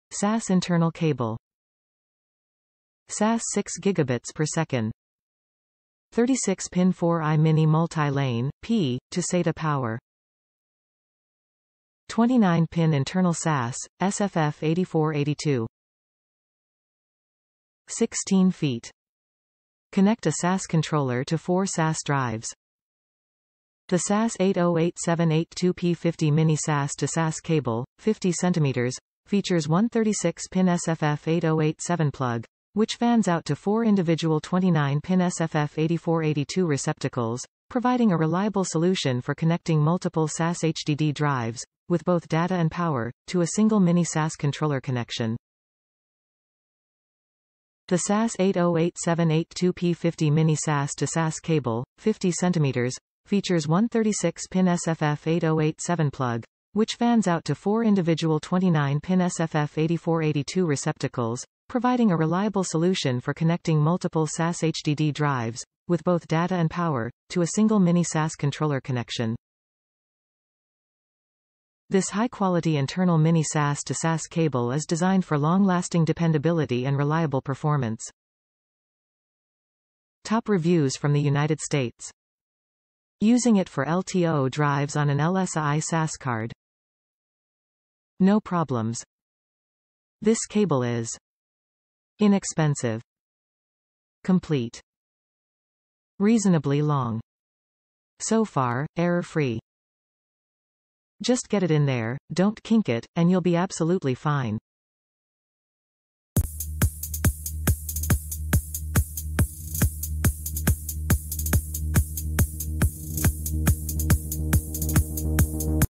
StarTech.com 50cm Internal Serial Attached SCSI Mini SAS Cable - SFF8087 to 4x SFF8482
SAS internal cable
SAS 6Gbit/s
36 pin 4i Mini MultiLane (P) to SATA power
29 pin internal SAS (SFF-8482)
16 ft
Connect a SAS Controller to 4 SAS drives. The SAS808782P50 Mini SAS to SAS cable (50cm) features one 36-pin SFF-8087 plug, which fans out to four individual 29-Pin SFF-8482 receptacles, providing a reliable solution for connecting multiple SAS HDD drives (with both data and power) to a single mini SAS controller connection..
The SAS808782P50 Mini SAS to SAS cable (50cm) features one 36-pin SFF-8087 plug, which fans out to four individual 29-pin SFF-8482 receptacles, providing a reliable solution for connecting multiple SAS HDD drives (with both data and power) to a single mini SAS controller connection.
This high quality internal Mini SAS to SAS cable is designed for long-lasting dependability and reliable performance.
Using it for LTO drives on an LSI SAS card. No problems.
This cable is:

Inexpensive
Complete
Reasonably long
So far, error-free

Just get it in there, don't kink it, and you'll be absolutely fine.